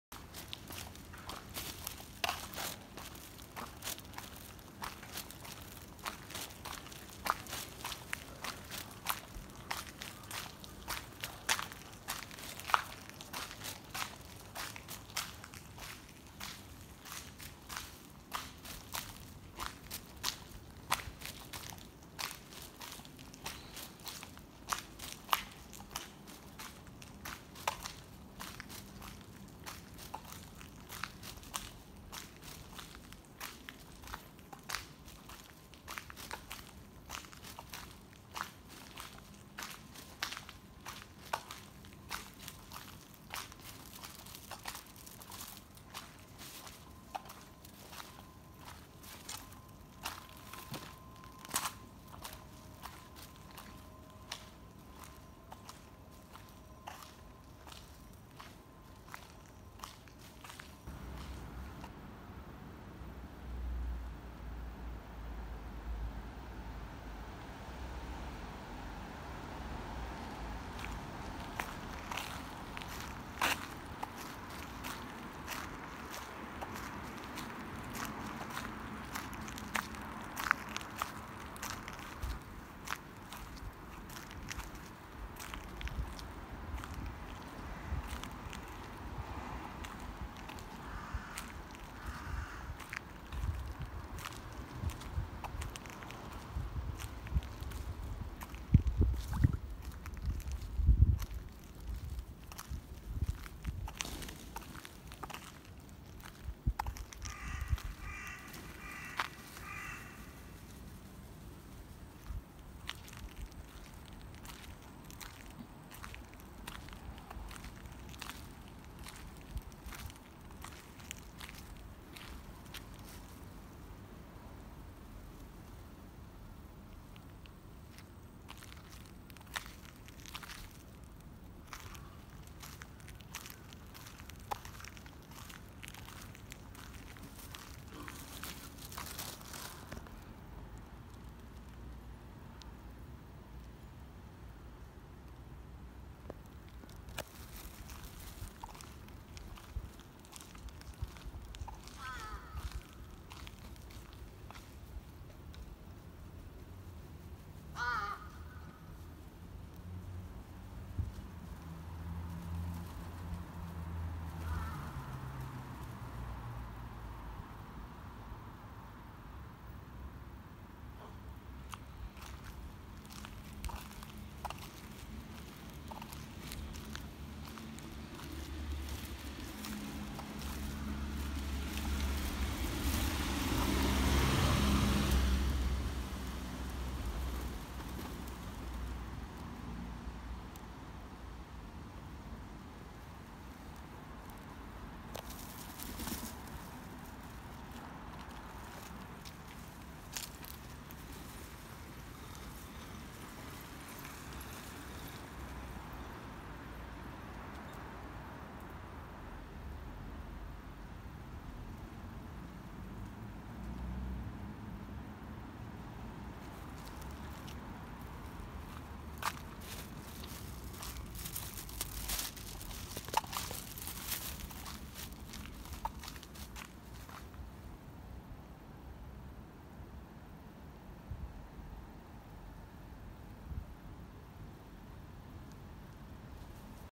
AM4:00雨上がりを歩いた
雨が止んで虫が出てきてるのかと思いきや...
糞害もあると思うので餌を撒くのはやめましょう